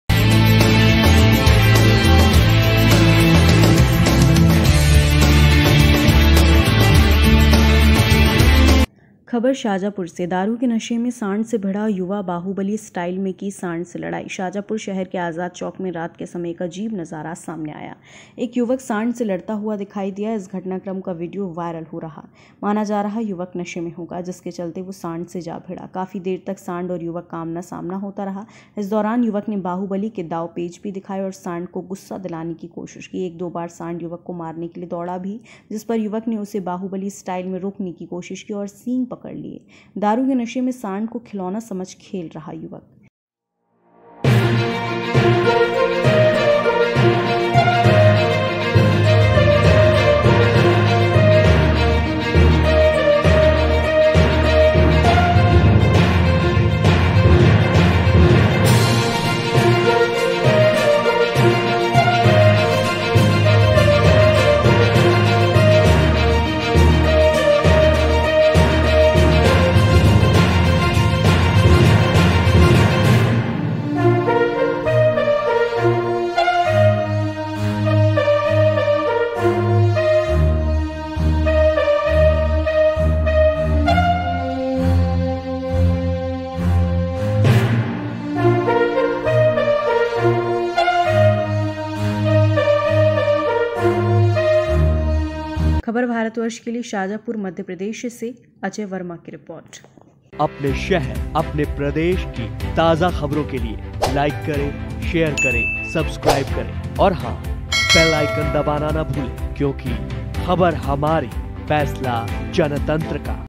दारू के नशे में सांड से भिड़ा युवा बाहुबली स्टाइल में की सांड से लड़ाई , वीडियो जमकर हो रहा वायरल
शाजापुर (मप्र) : दारू के नशे में सांड से भिड़ा युवा बाहुबली स्टाइल में की सांड से लड़ाई , घटनाक्रम का वीडियो जमकर हो रहा वायरल